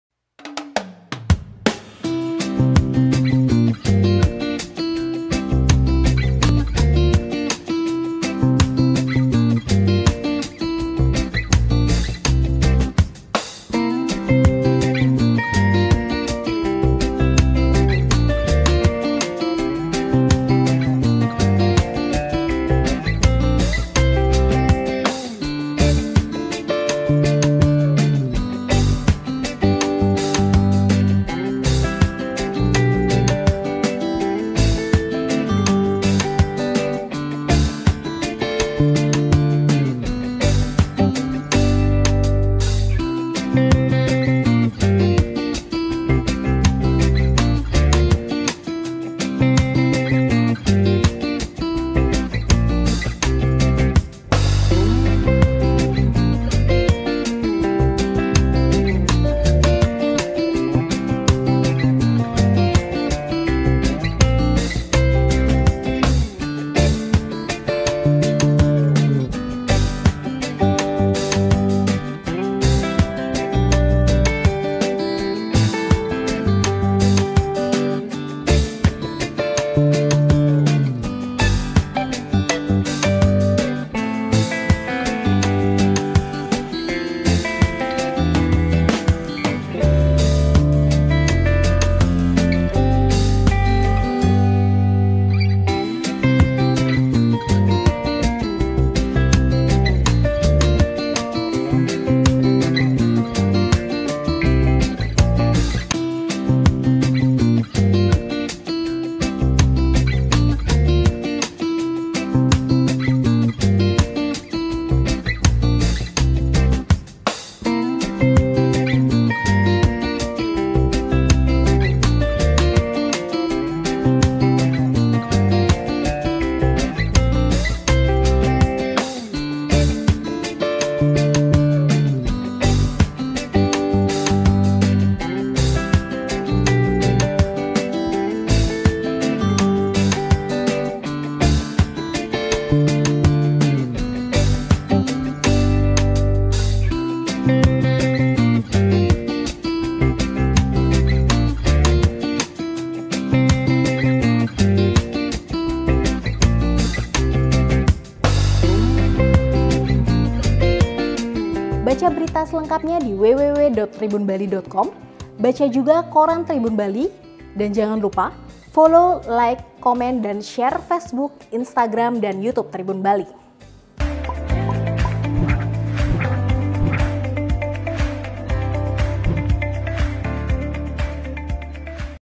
SKB CPNS Dimulai Esok, Diikuti 4.356 Peserta untuk Seluruh Formasi di Bali
Selasa (1/9/2020) esok pelaksanaan Seleksi Kompetensi Bidang (SKB) CPNS tahun 2019 mulai digelar.
Tes ini akan dimulai untuk formasi pada tingkat provinsi Balu yang digelar selama 3 hari hingga tanggal 3 September 2020.
Selanjutnya akan disusul pelaksanaan SKB CPNS untuk kabupaten/kota se-Bali.

(IAG)